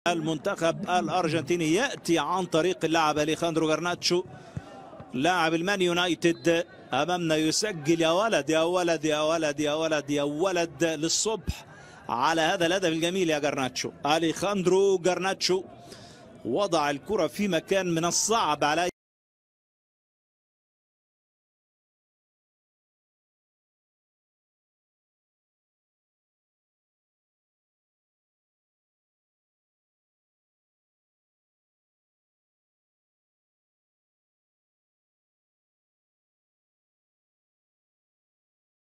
Alejandro Garnacho - Amazing freekick goal vs U20 France
►The GOAT: Strong Legs, Clear Mind

Alejandro Garnacho goal
U20 Argentina - U20 France